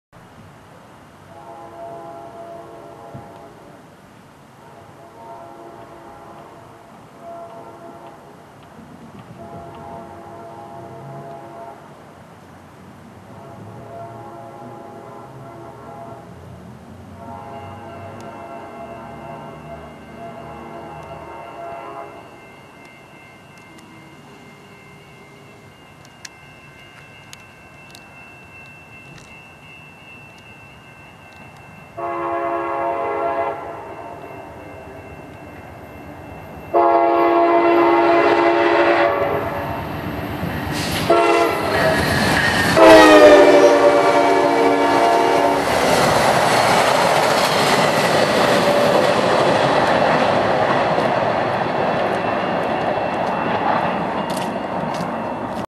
P42 916 Leads VIA 61 Through Brockville
VIA 61 passes by Brovckville on the South track with P42 916 and some LRC cars on March 9th, 2013.